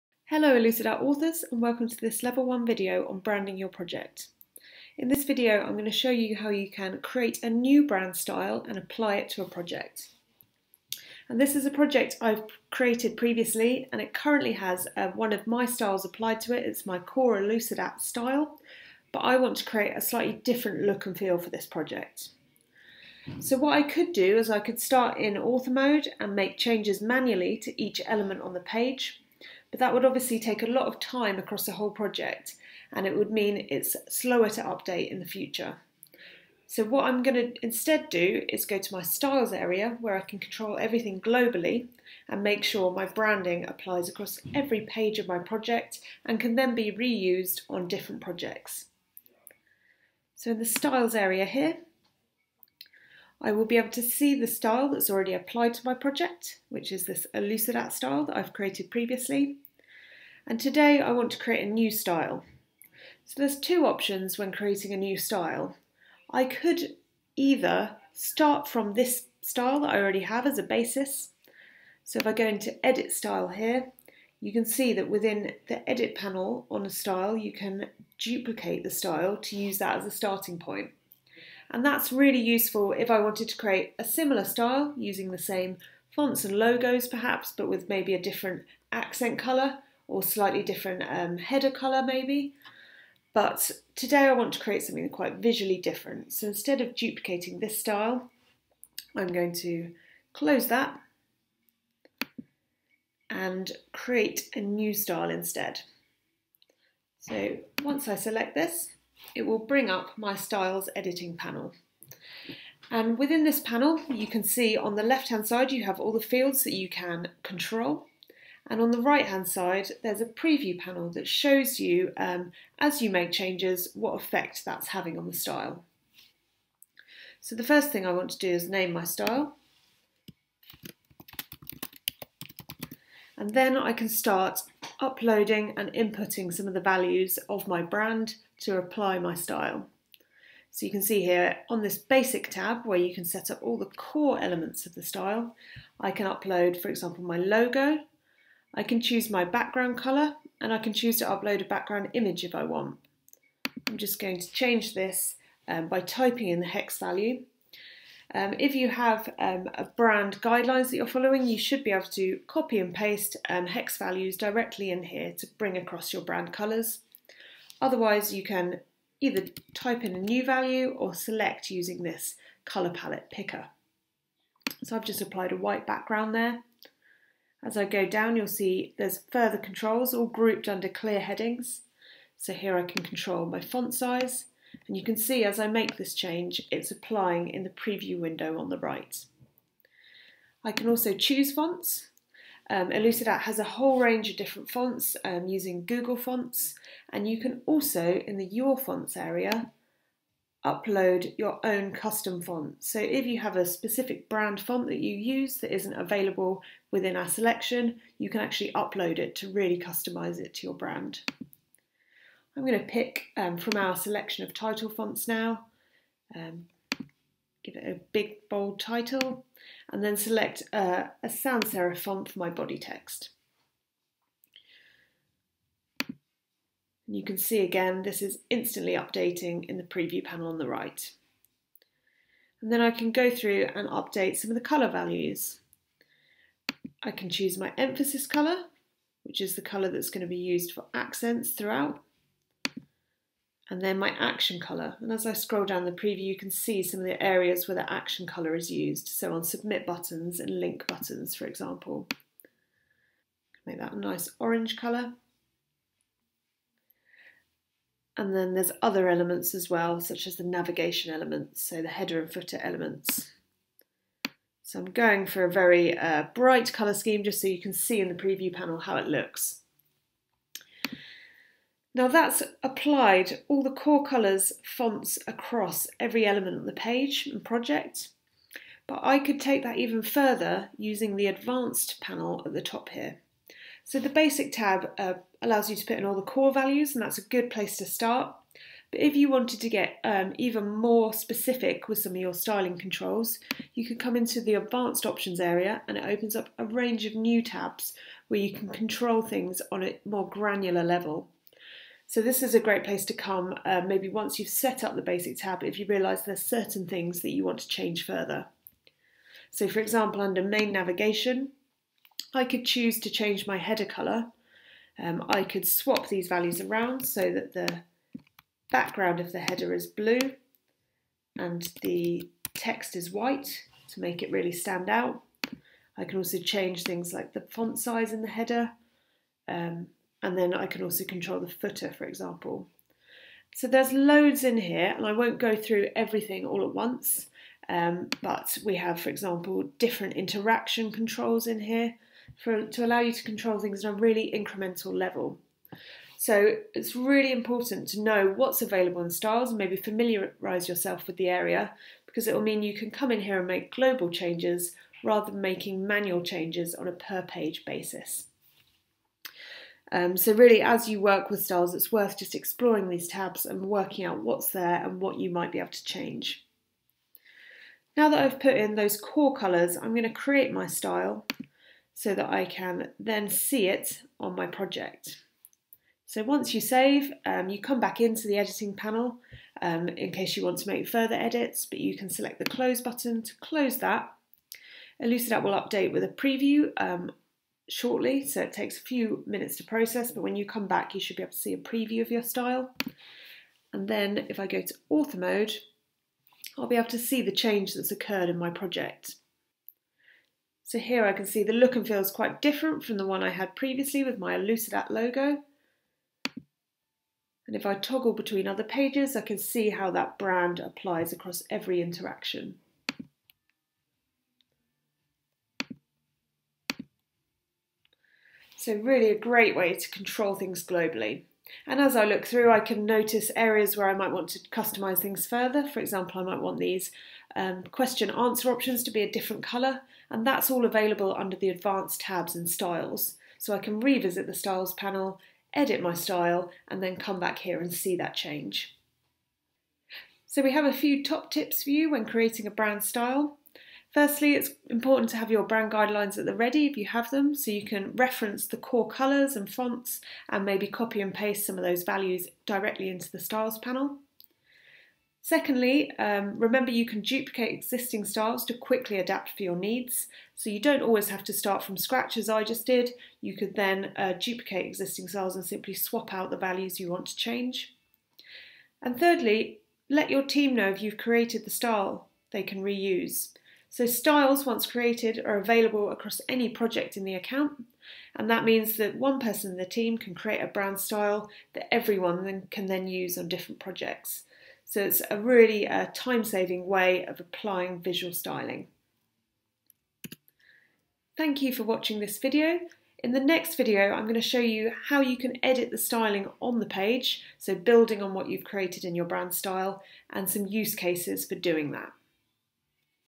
Create a brand style
The advanced features in the styling area give you a lot of control over the look and feel of your project. Reinforce your knowledge and see some working examples in this masterclass:

Looking for more info on how to save and share your styles? Check out this support article: